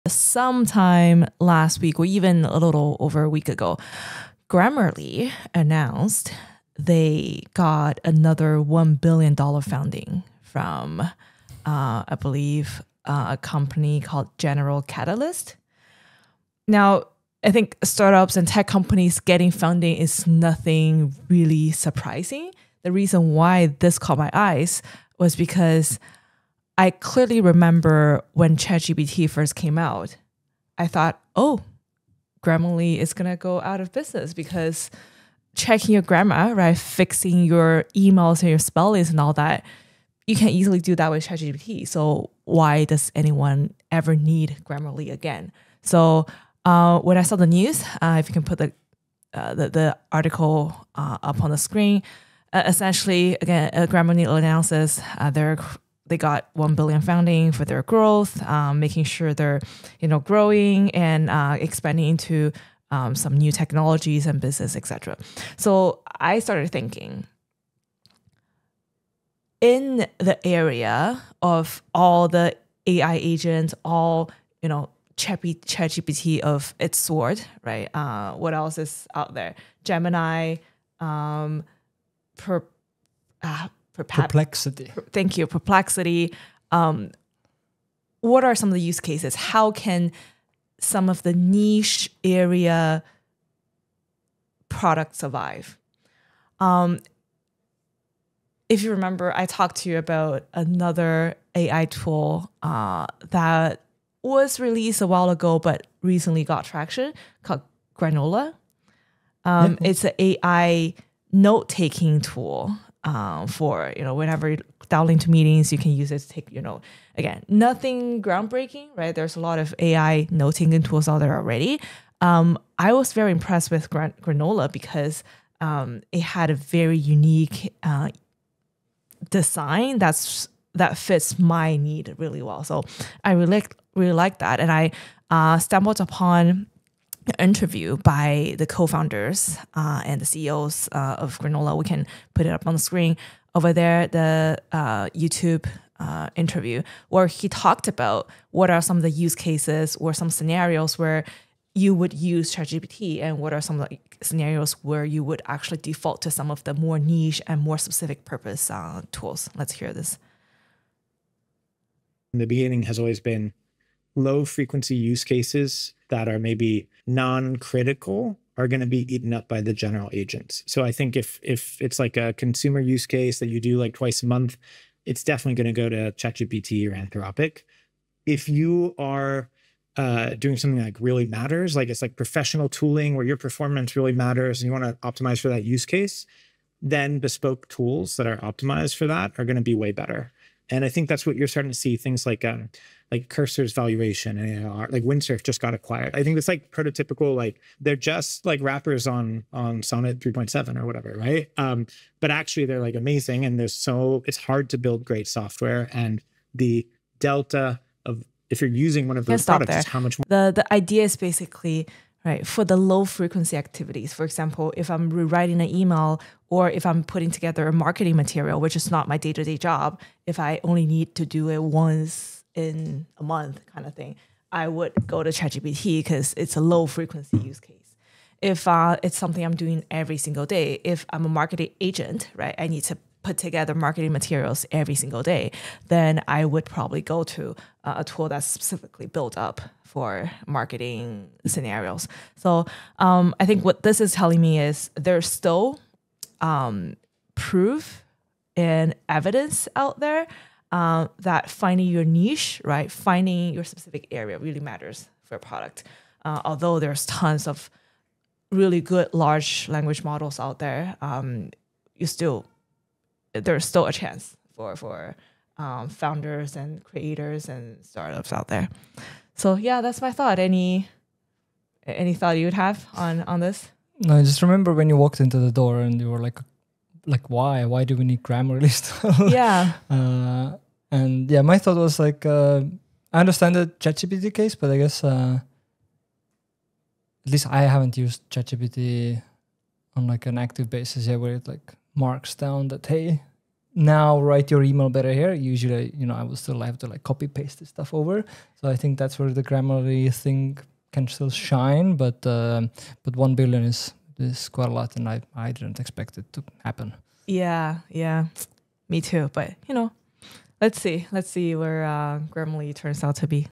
Grammarly Just Raised $1B... But Isn’t ChatGPT Supposed to Kill It?
World dominated by AI giants like ChatGPT, Gemini, and Perplexity, how is a grammar-checker still thriving? Grammarly managed to secure another $1 billion in funding.

* Why niche AI tools like Grammarly and Granola are surviving (and thriving)?
* What top AI founders think about generalist agents vs specialized tools?
* How frequency of use affects AI product adoption?
* What this means for the future of startups in the age of LLMs?

How many AI tools are you using? Is it just ChatGPT or are there a bunch of other tools in there as well? 👀